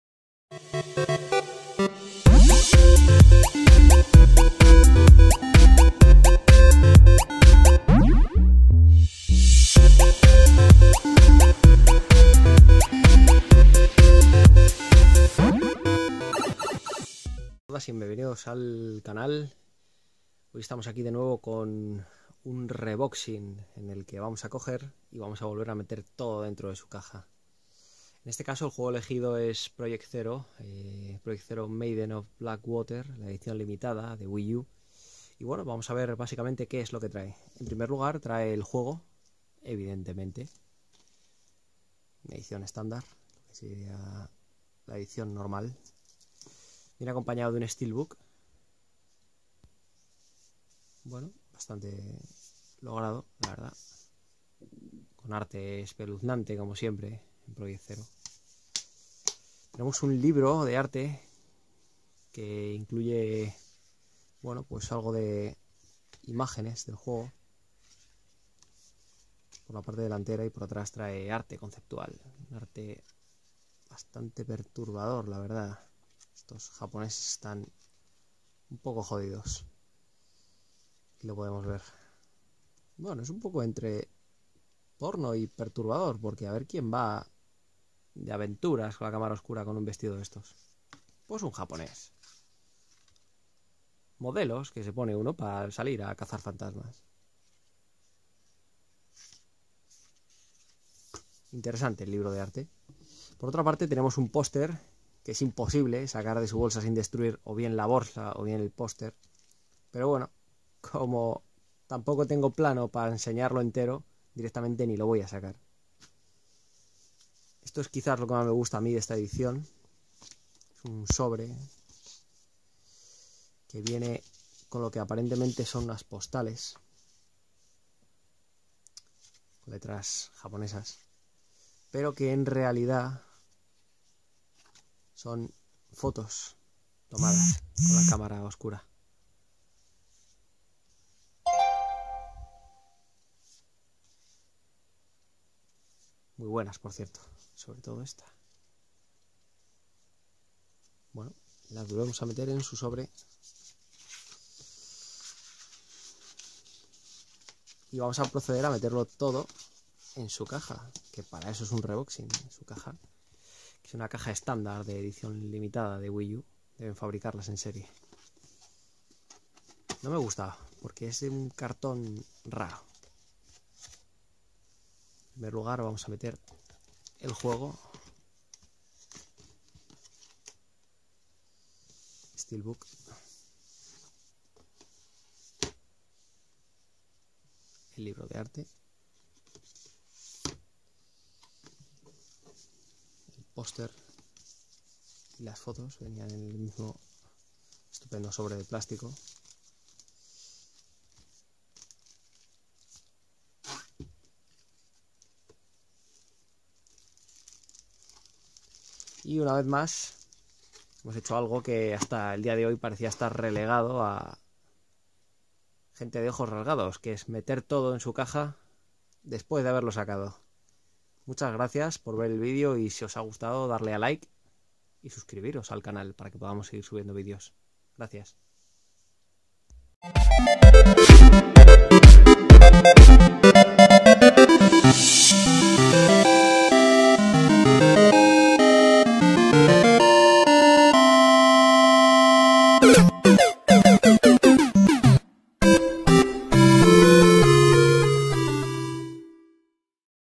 Unboxing Project Zero Maiden of Black Water Limited Edition Wii U
De nuevo os traemos un "reboxing", de otra edición limitada en este caso de Project Zero Maiden of Black Water. Y aunque meter las cosas en sus cajas después de sacarlas sea un "trabajo de chinos", ¡lo hemos conseguido!